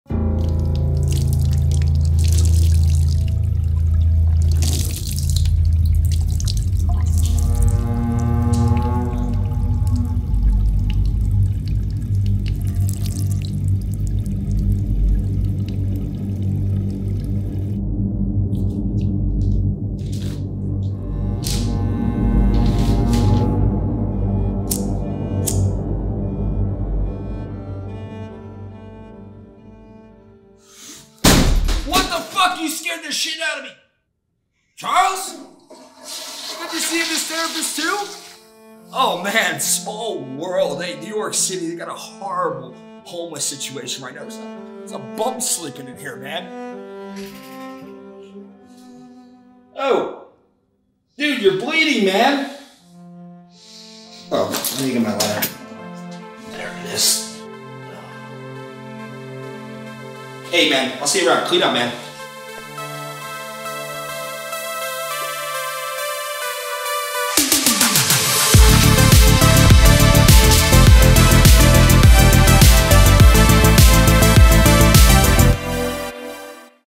Charles Mason Meets Joey Janela | HOG Short
Shot and edited by Micael Preysler.
Featuring Charles Mason & Joey Janela.
Shot on Blackmagic Pocket CC 6K & Zeiss Contax.